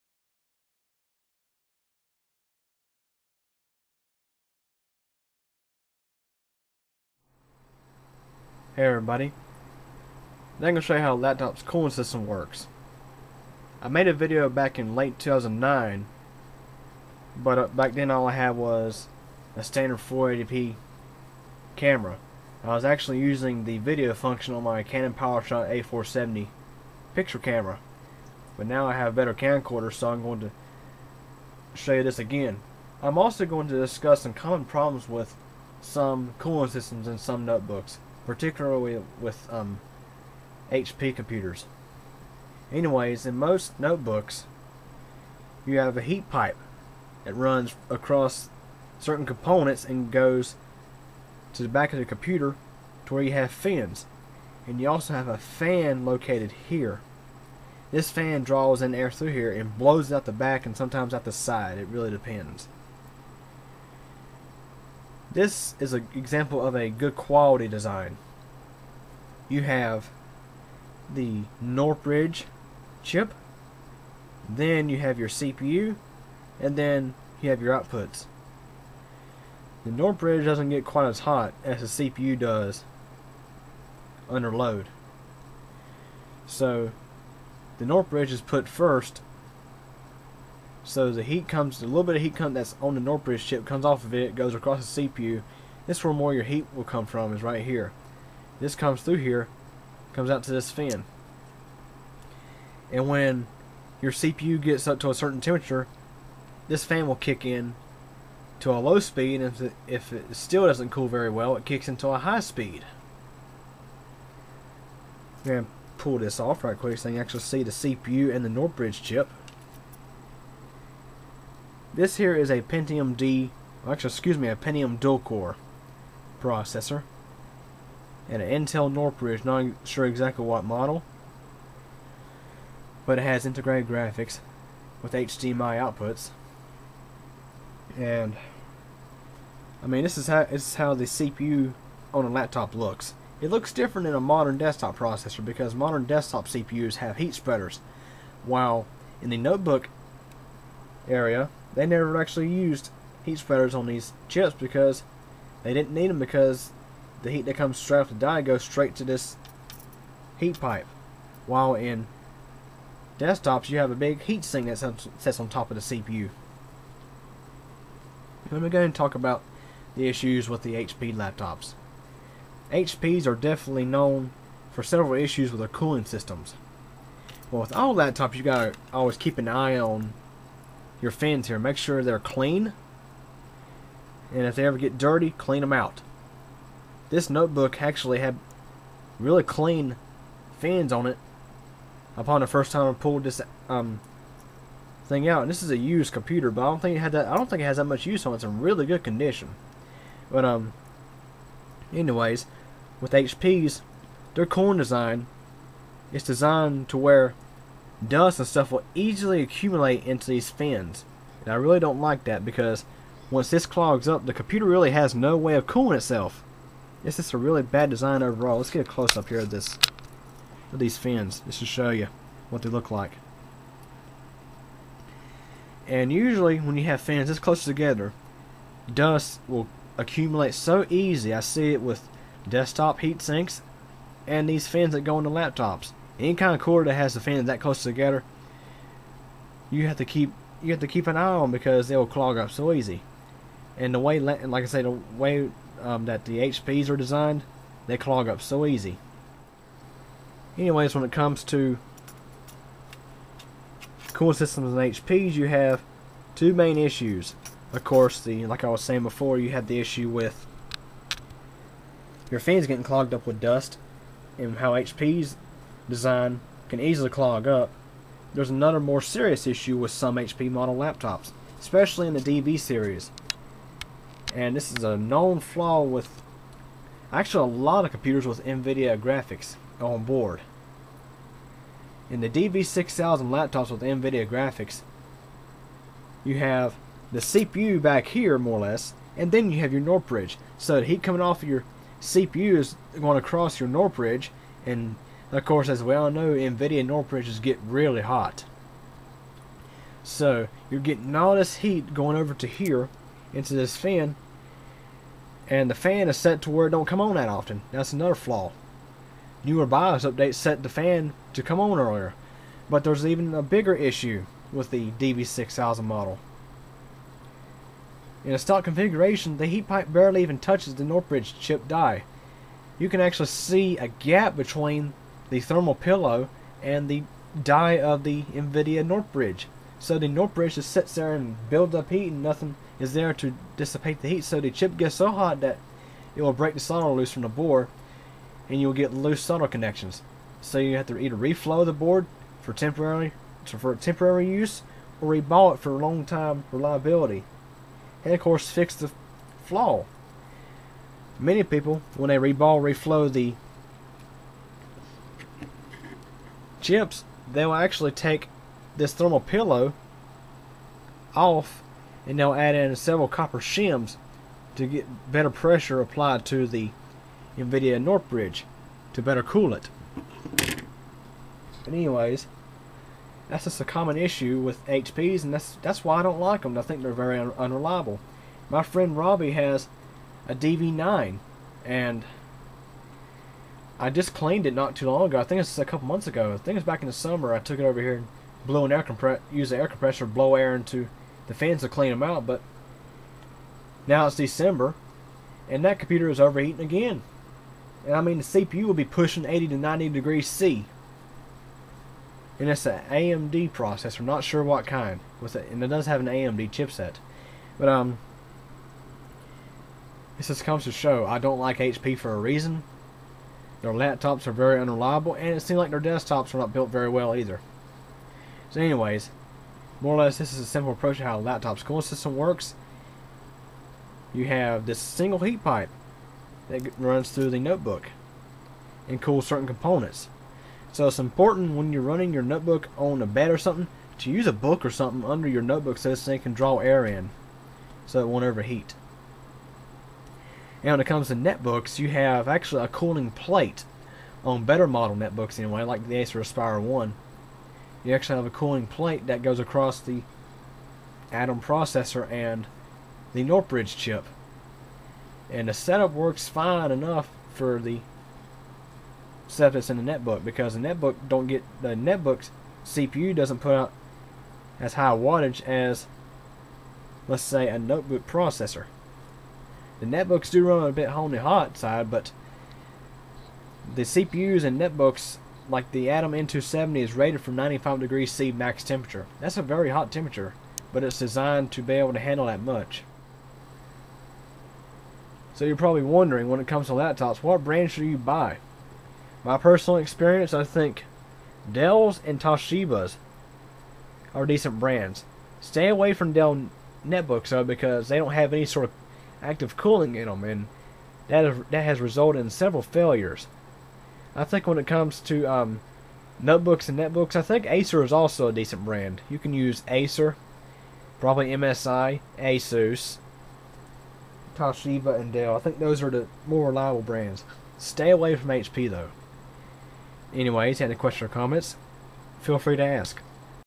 How a Laptop's Cooling System Works - Episode II
In this video, I explain the concept of how a notebook's cooling system works. I also explain why I don't like HP/Compaq notebooks for the simple fact that they have had some serious issues with the cooling systems in some of their notebooks. By watching this, you'll also learn why it is really important to keep your notebook's cooling system free of dust, etc, and to allow the intake on the bottom side of the notebook to get air at all times.